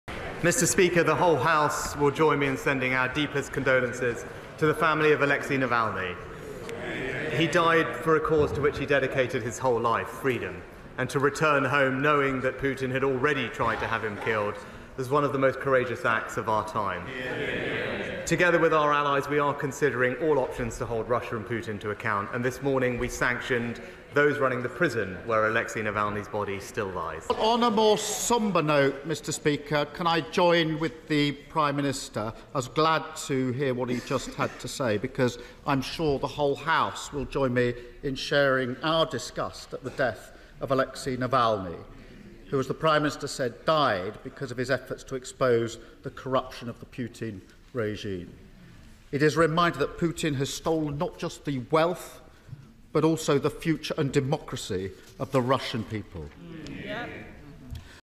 Sunak and Starmer pay tribute to Alexei Navalny after Putin critics death
Rishi Sunak has confirmed the UK and its allies are "considering all options" to hold Russia and Vladimir Putin to account after the death of Alexei Navalny.

He made the comments during a session of Prime Minister's Questions on Wednesday 21 February.

"[Navalny] died for a cause to which he dedicated his whole life - freedom," Mr Sunak said, calling his decision to return to Russia "one of the most courageous acts of our time".

Sir Keir Starmer also paid tribute to Navalny and shared his "disgust" over the 47-year-old's death.